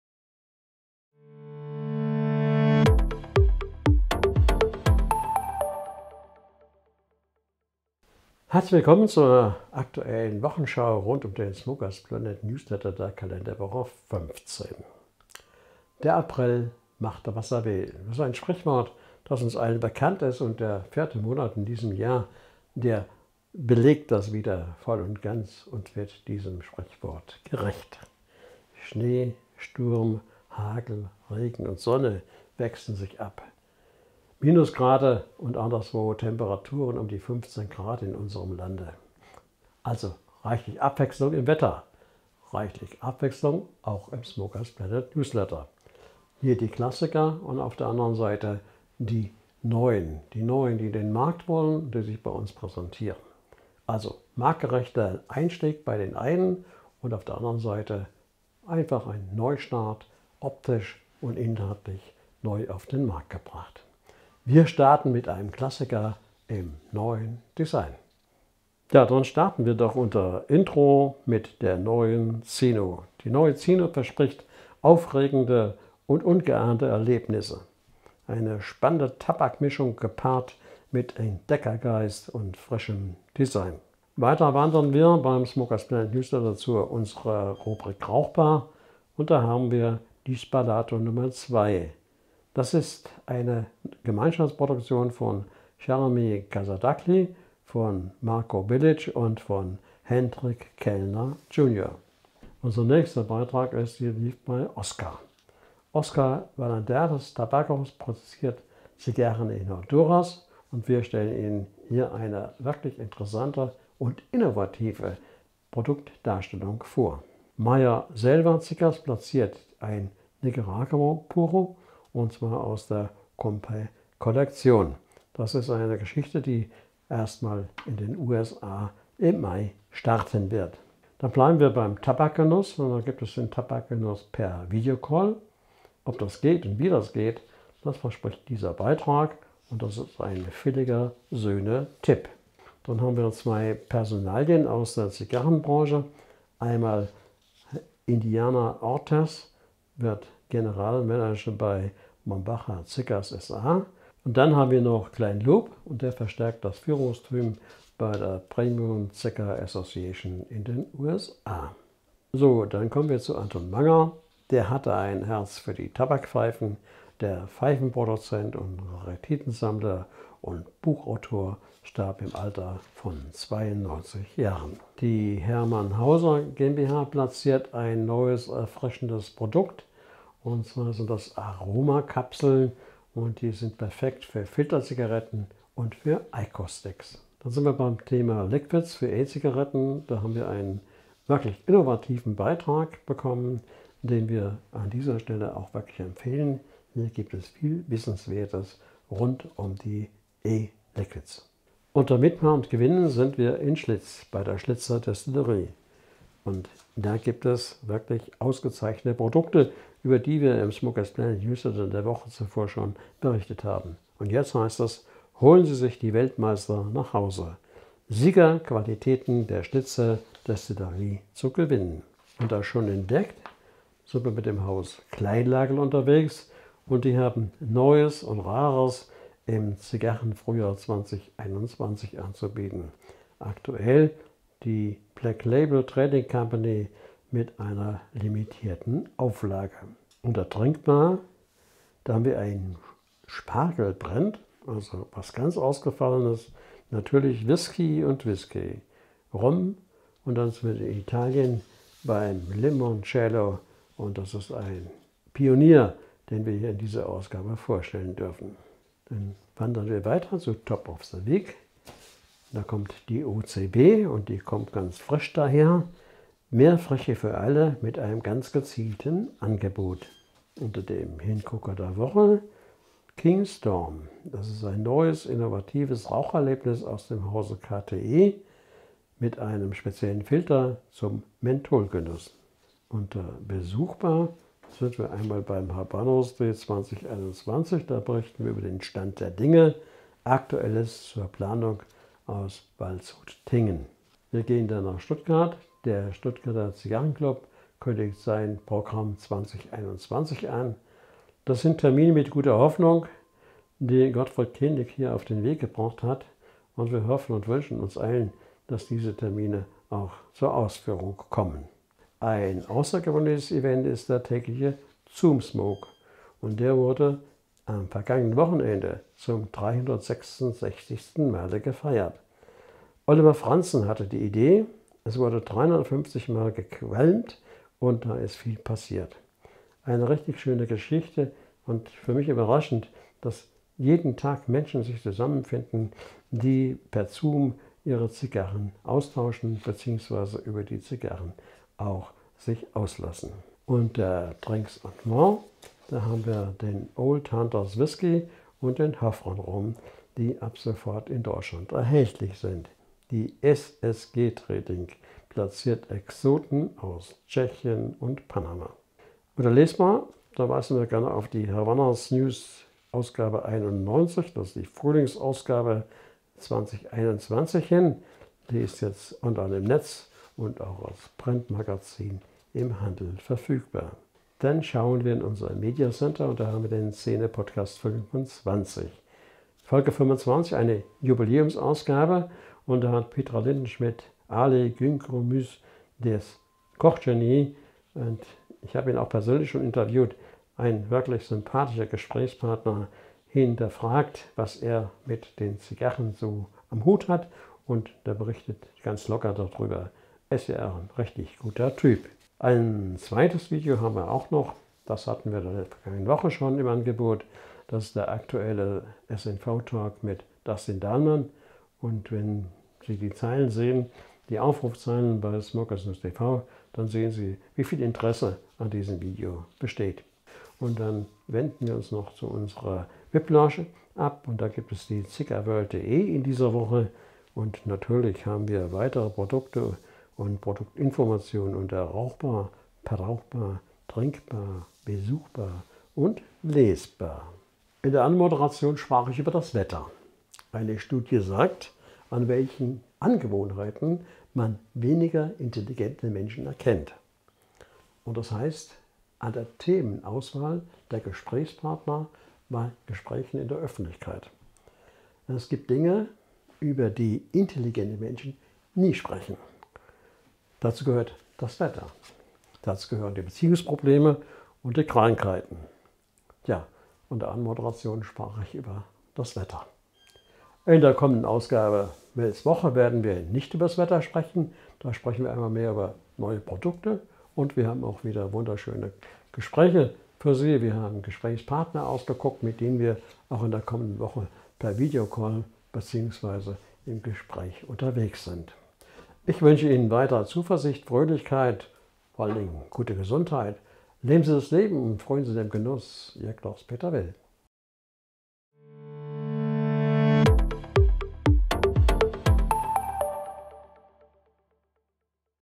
Will´s Woche KW 15/2021
Der April macht was er will und bietet reichlich Abwechslung im Wetter - und so ist dies auch im sp-Newsletter der KW 15: Klassiker und Newcomer geben sich die Tür in die Hand und sorgen für Abwechslung. Also wir bleiben wie immer informativ und schauen, was wir Ihnen in Will´s Woche aus dem wöchentlichen Nachrichtenticker "smokersplanet.de" offerieren können.